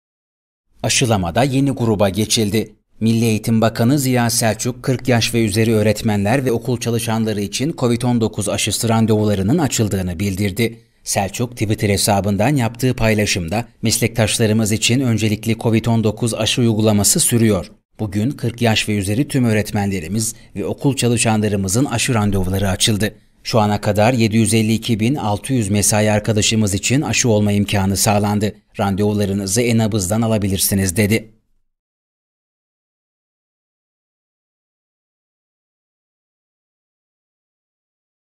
AŞILAMADA YENİ GRUBA GEÇİLDİ / 40 Yaş Üzeri Öğretmenler İçin Aşı Randevuları Açıldı
AŞILAMADA YENİ GRUBA GEÇİLDİ
40 Yaş Üzeri Öğretmenler ve Okul Çalışanlarının Aşı Randevuları Açıldı
40 yaş üzeri öğretmenler ve okul çalışanları için aşı randevuları açıldı. Milli Eğitim Bakanı Ziya Selçuk, "Şu ana kadar 752 bin 600 mesai arkadaşımız için aşı olma imkanı sağlandı." dedi.
Aşılamada yeni gruba geçildi.  Milli Eğitim Bakanı Ziya Selçuk, 40 yaş üzeri öğretmenler ve okul çalışanları için Kovid-19 aşısı randevularının açıldığını bildirdi.
Selçuk, Twitter hesabından yaptığı paylaşımda, "Meslektaşlarımız için öncelikli Kovid-19 aşı uygulaması sürüyor. Bugün 40 yaş üzeri tüm öğretmenlerimiz ve okul çalışanlarımızın aşı randevuları açıldı. Şu ana kadar 752 bin 600 mesai arkadaşımız için aşı olma imkanı sağlandı. Randevularınızı e-Nabız'dan alabilirsiniz." Dedi.